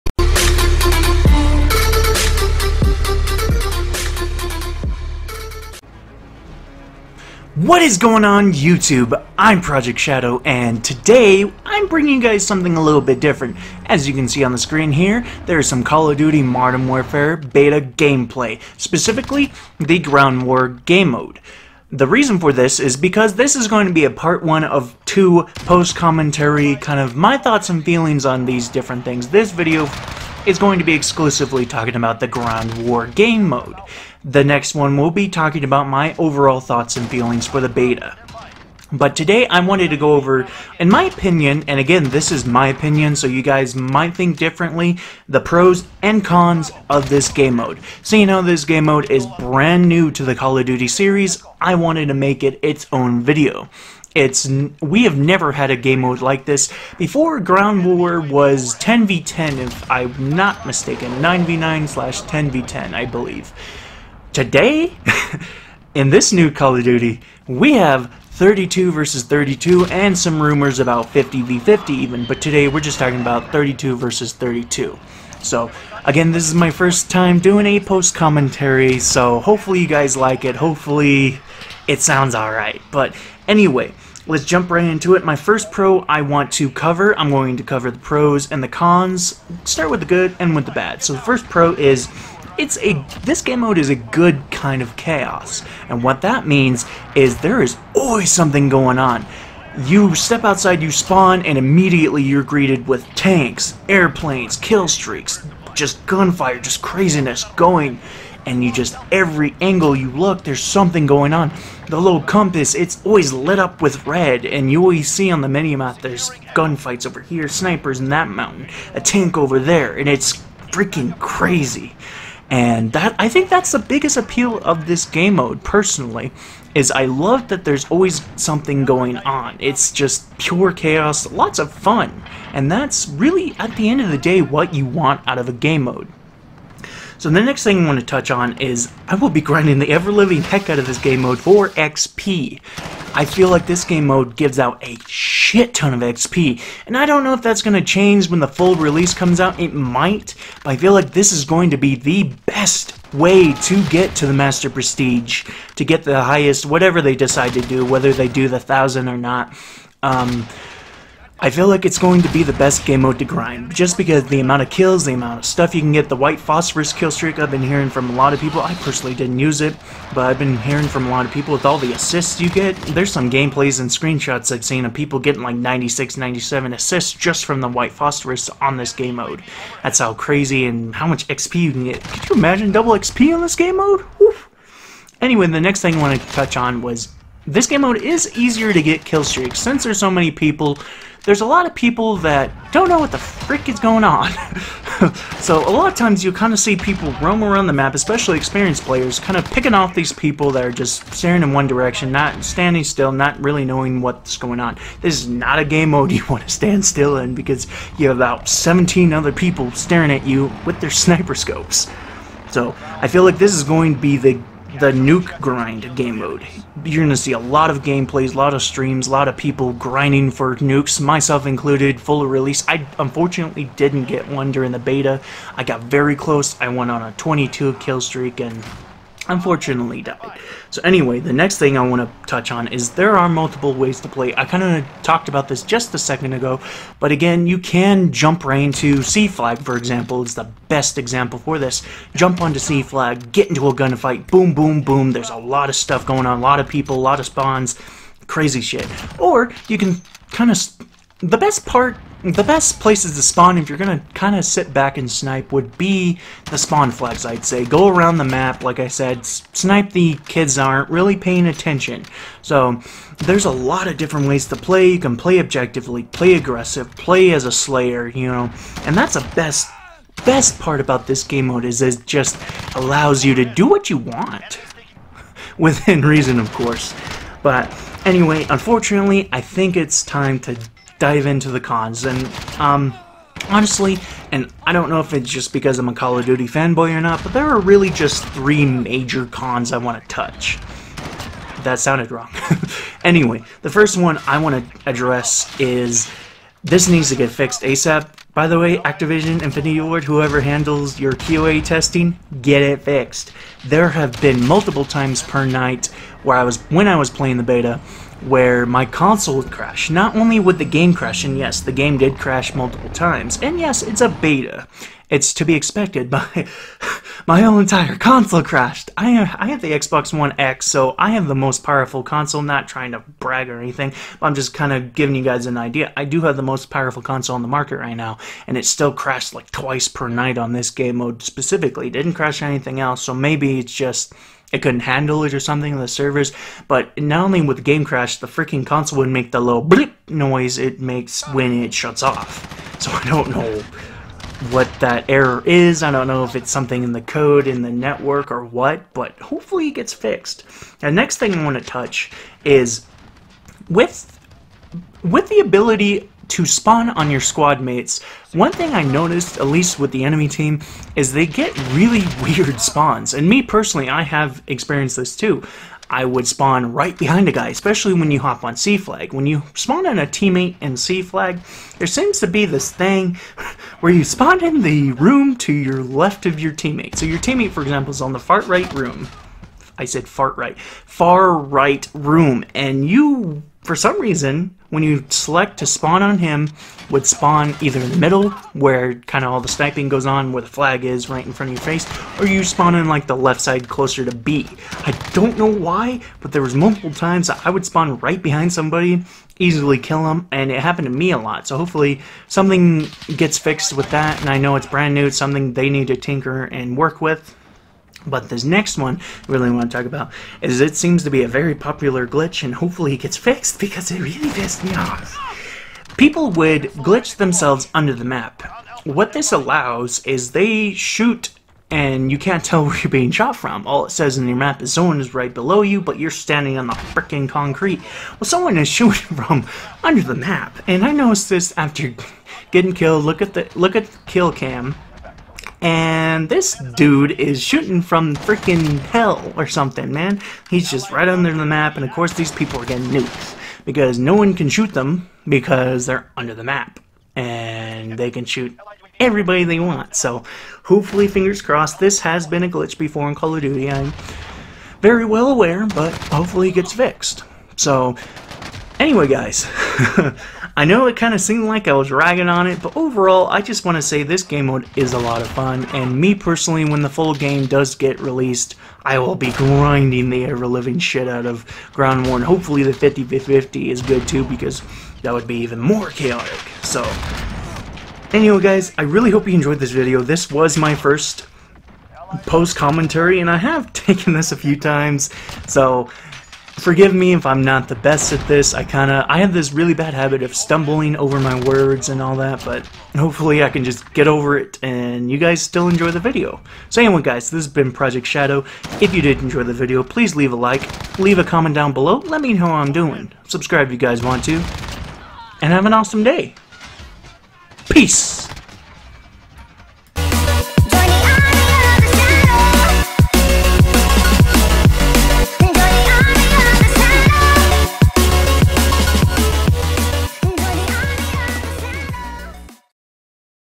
My Thoughts on Modern Warfare Ground War
Is the new ground war game mode any good? Come find out what I think!